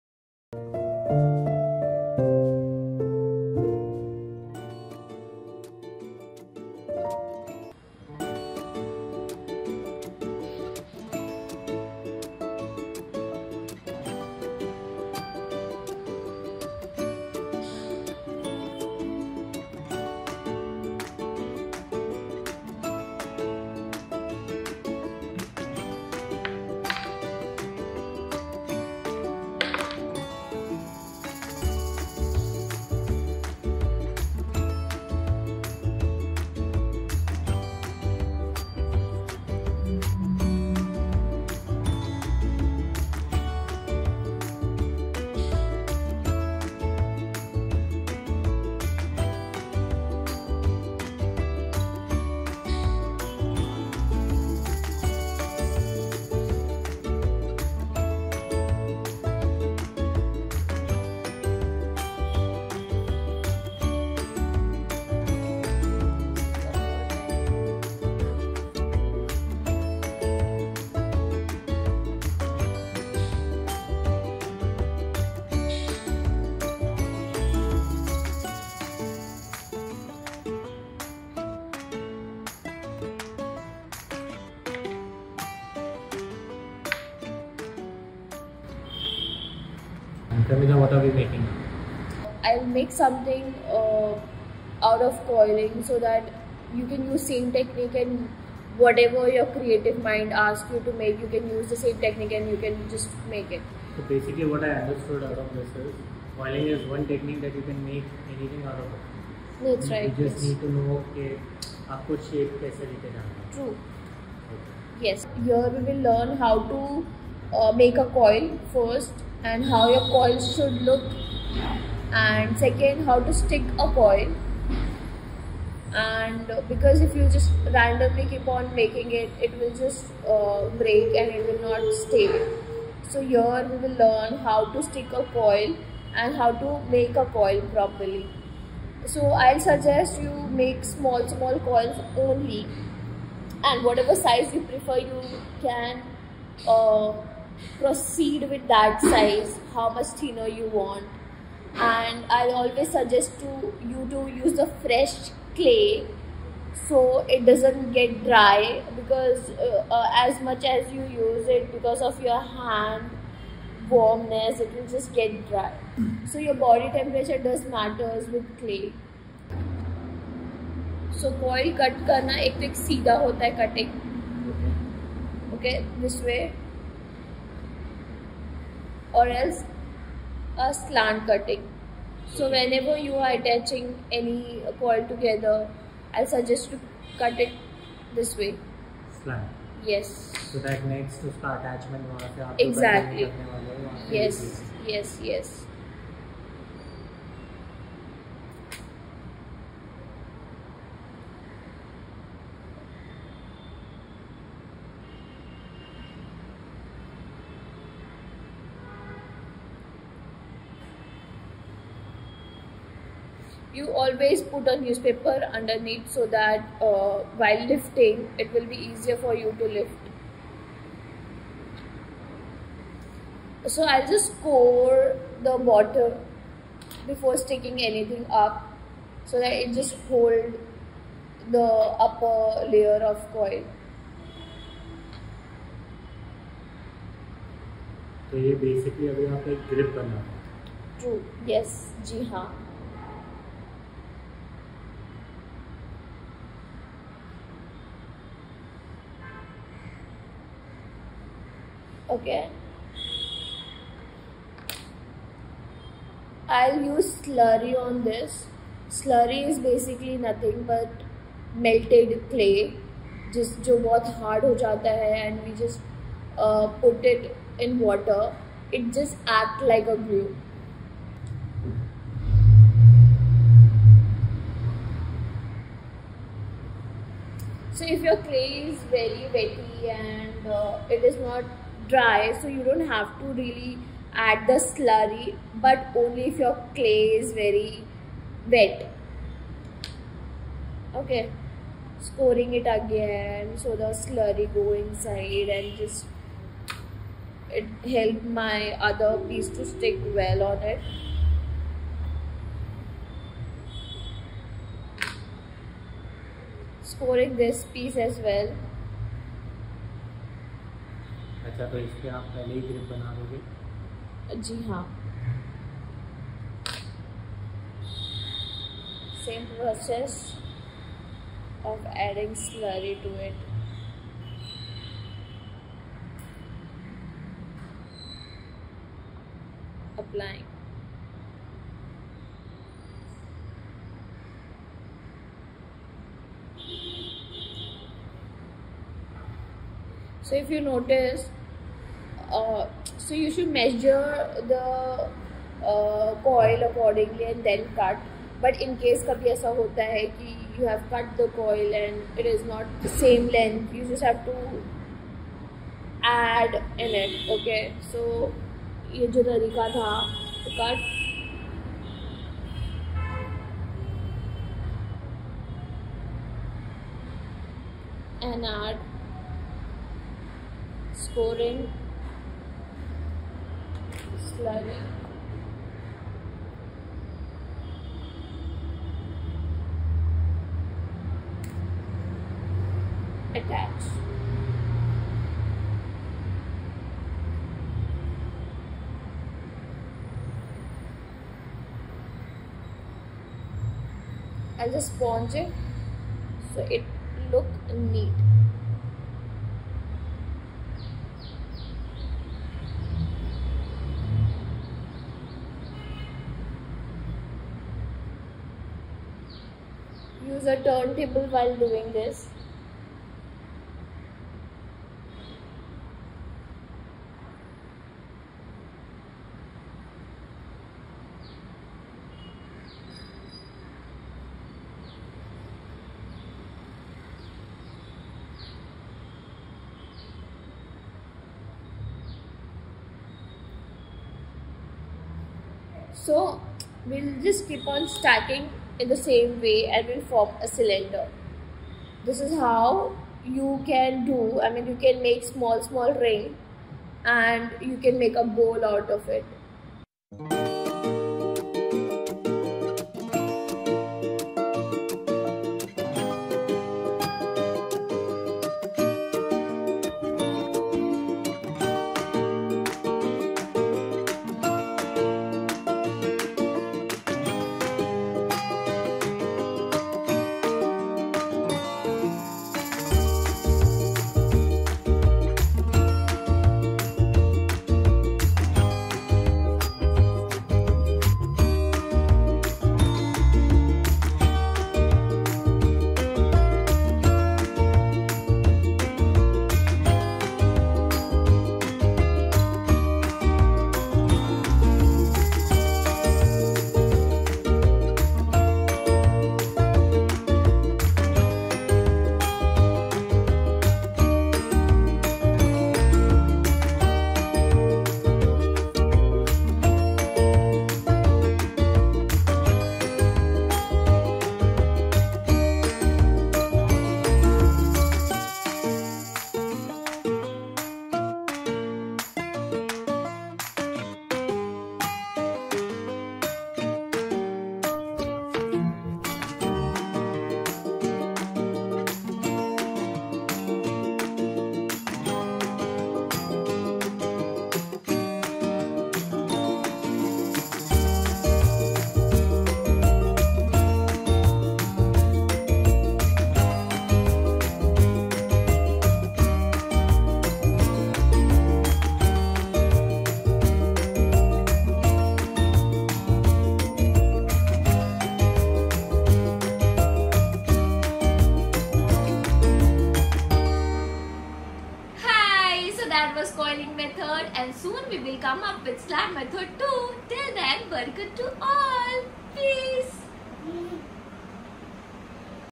Tutorial on Coiling Technique, Chapter #3
Coling technique is one of the handmade technique of pottery, i tried explaining in details , do comment if have any question.

For chapter 1,2,3 check my videos

or search for barkartart